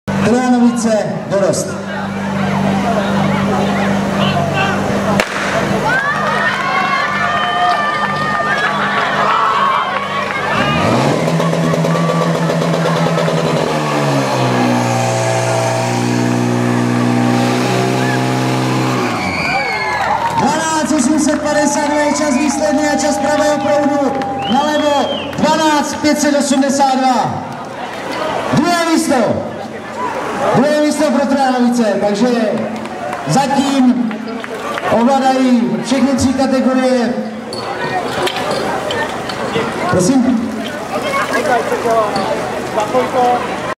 Trojanovice dorost Vyšní Lhoty 26 6 2020
7. ročník Poháru FIRE TV a Poháru hory Prašivé Vyšní Lhoty okr.FM/ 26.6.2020